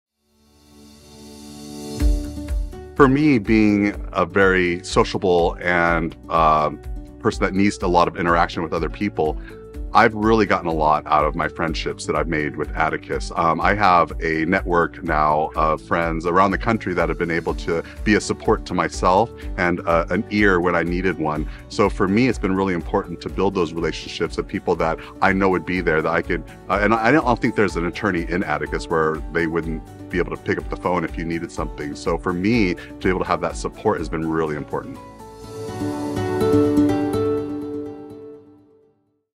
Atticus® Success Story- Sam Price Testimonial 02
Sam Price, Estate Planning, Trusts, and Probate, is a current Atticus member and has learned how to create a great practice and build a great life. For him, this includes being a great father, leader, and mentor. He is choosing to live his life purposefully.